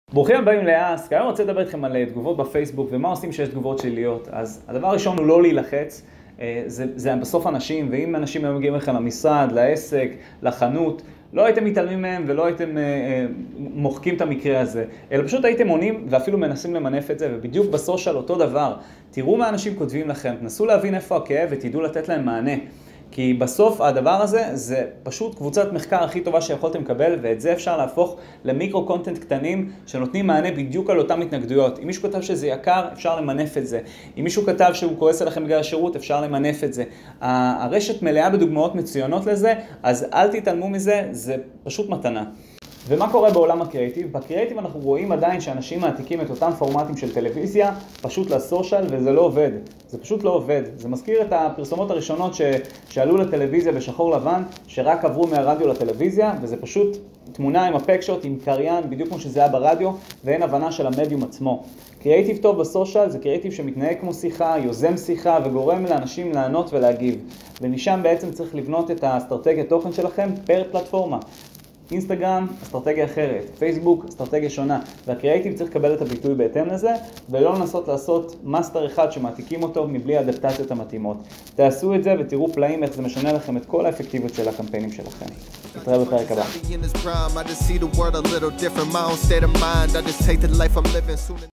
2 טעויות הנפוצות בסושייל ואיך להמנע מהן
דיגיטל או סושייל - אלו מילים מאוד משטיחות.
כל מילה כזו מכילה בתוכה סאב התמחויות שחשוב להבין את ההבדל בינהן ואת האסטרטגיה השונה שצריך לכל פלטפורמה.

2 טעויות נפוצות שמונעות מהרבה אנשים להצליח בסושייל הן:
1. תקשורת בינאישית ואופן יצירת המעורבות
2. הקראיטיב והתאמתו למדיום.
* לינק לפרסומת הראשונה שנעשתה בטלוויזיה.